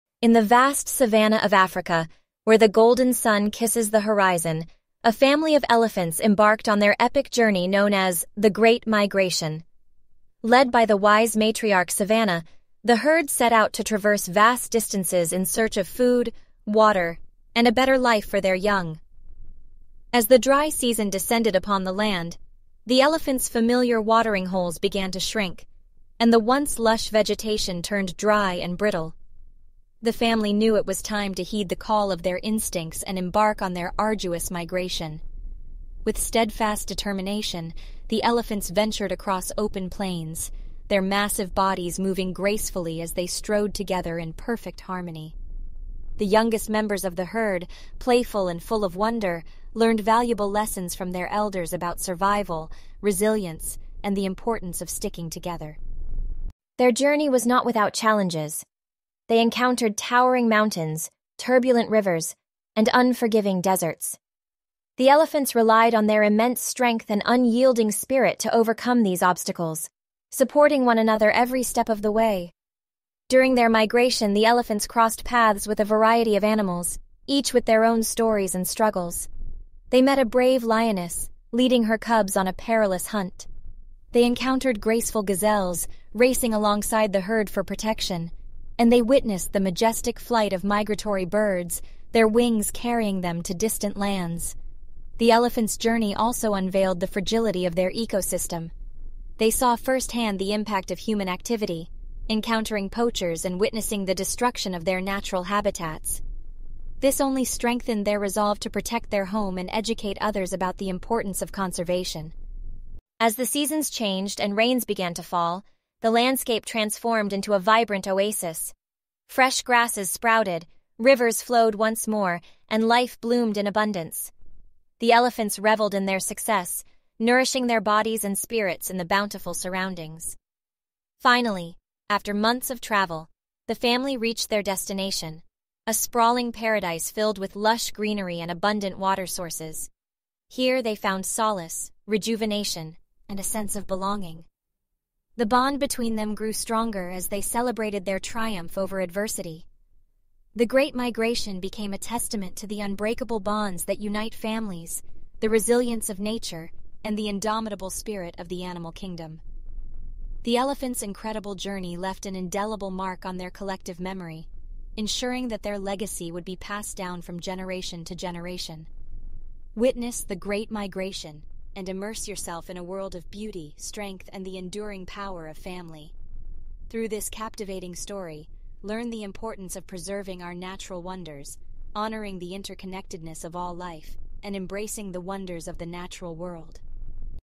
A Journey of Giants: The Epic Tale of The Great Elephant Migration
Embark on an awe-inspiring adventure with "A Journey of Giants: The Epic Tale of The Great Elephant Migration." Follow a remarkable family of elephants as they overcome challenges, traverse vast landscapes, and demonstrate the unbreakable bonds that unite them. This captivating story immerses readers in the beauty of the African wilderness, highlighting the resilience of these gentle giants and the importance of conservation. Discover the power of family and witness the extraordinary journey of these majestic creatures as they navigate through a changing world.